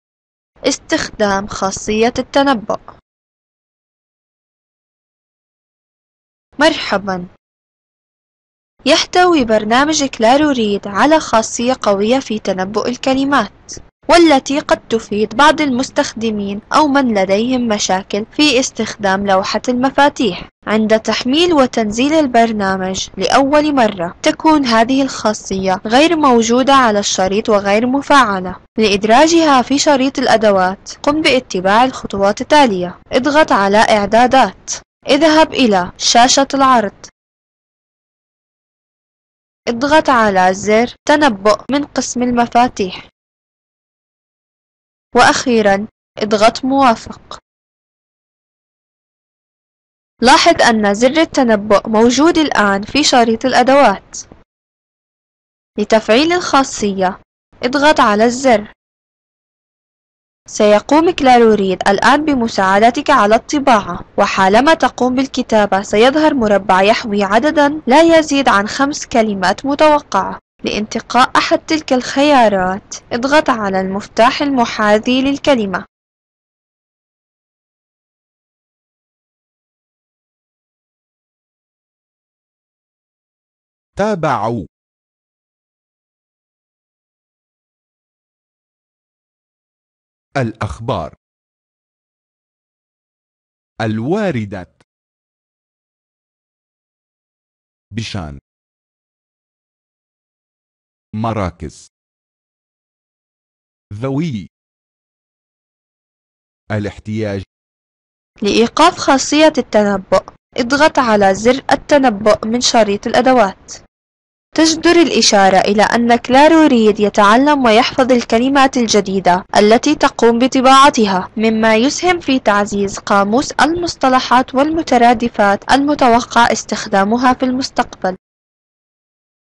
8 استخدام خاصية التنبؤ
خاصية التنبؤ في برنامج الكلارو ريد تقوم بتنبؤ بعدد من الكلمات أثناء الكتابة لتساعد على كتابة اسرع خاصة من لديهم عسر في الكتابة